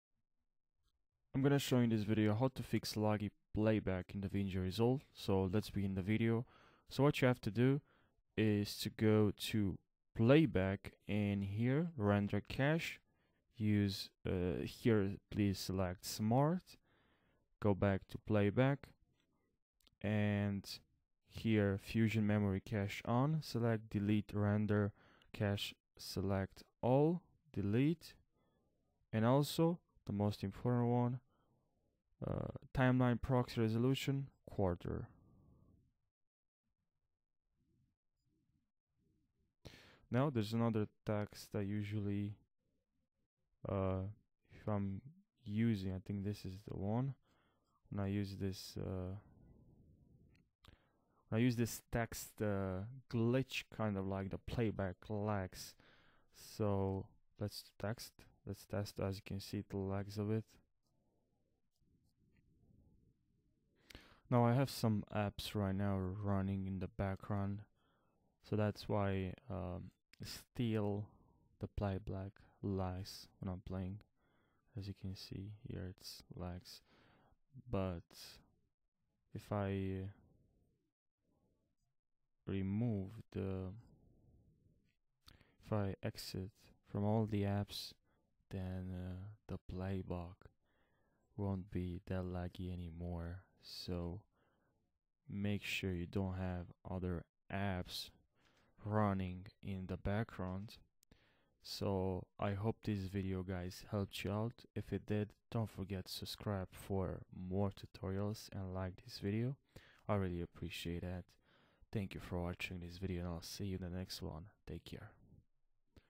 How To Fix Laggy Playback In DaVinci Resolve 18
You won't want to miss a single detail of this tutorial since I will take you step-by-step through the entire procedure. So please follow along with me and watch the entire video on How To Fix Laggy Playback In Davinci Resolve 18

Timestamp:
00:00 Intro
00:34 Start